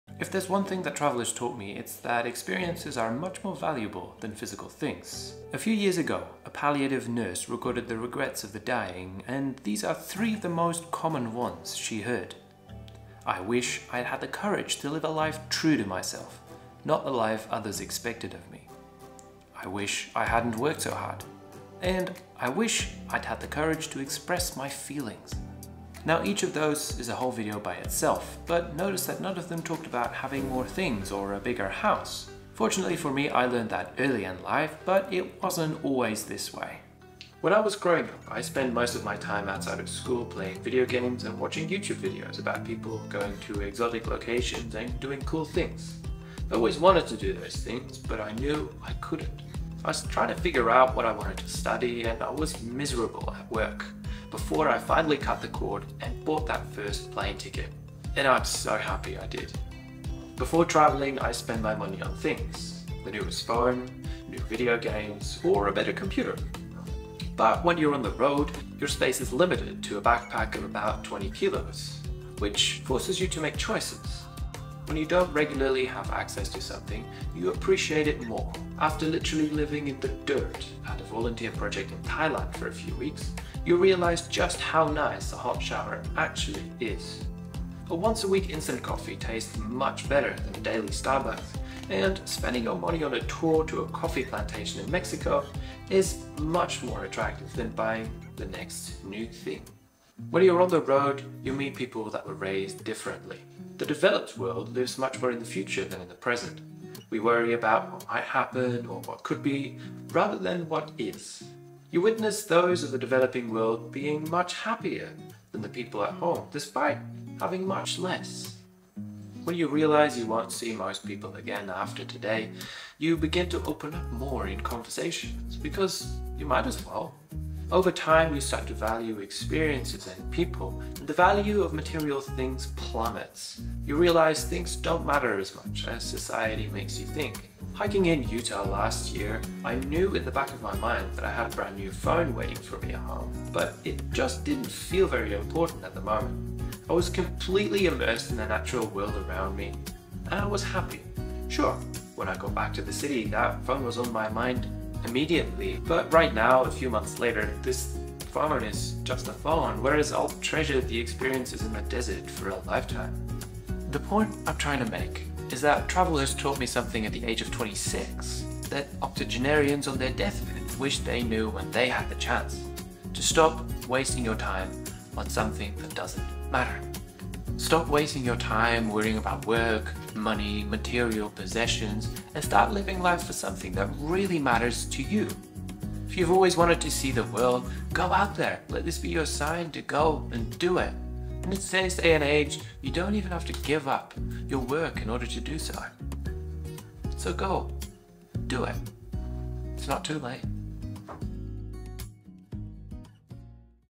The MOST VALUABLE LESSON | Life lesson from a Traveler
If there's one thing you should learn in life it's this.

Michel shares the #1 take-away he has from traveling the world: experiences are much more valuable than material things.